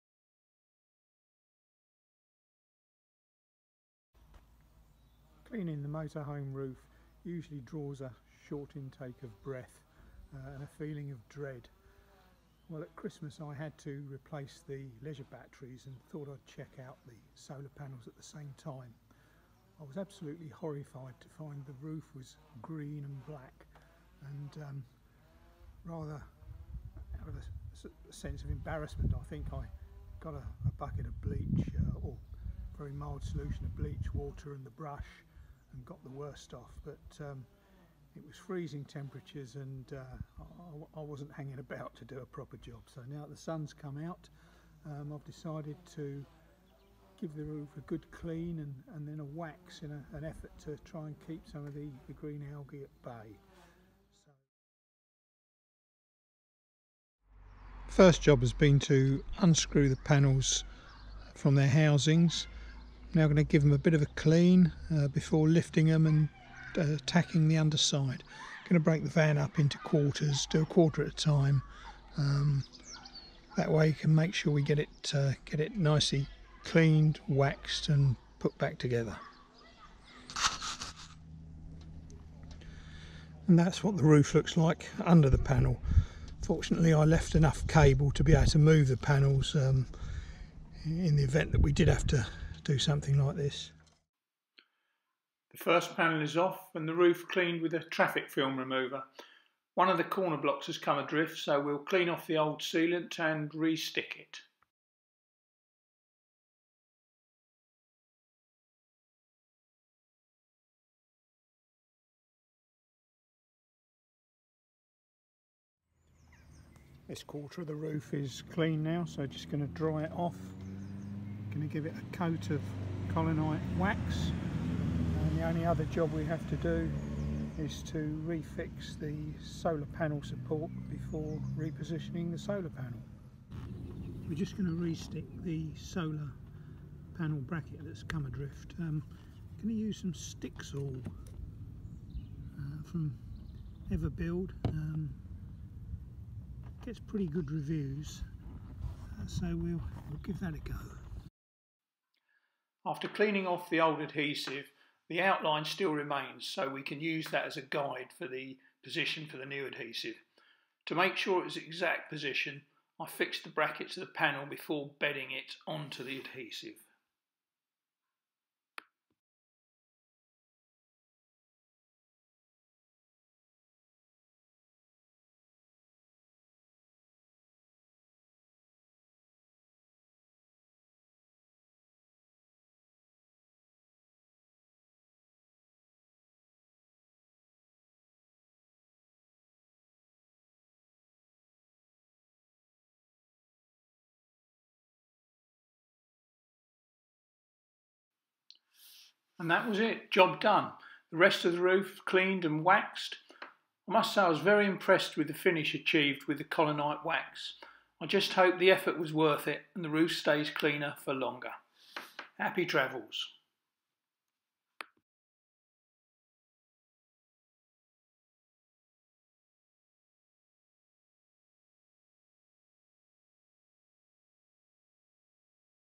Cleaning The Motorhome Roof HD 1080p
Cleaning the motorhome roof is not a job I relish so following a forum tip I thought I would clean with traffic film remover and give it a coat of wax polish in an effort to keep the dirt and algae at bay. Navigating the roof furniture was a challenge but most of the work could be completed using a step ladder. When on the roof I used a spreader board to avoid any point loads. Very impressed with the Collinite wax, no connection just a happy camper.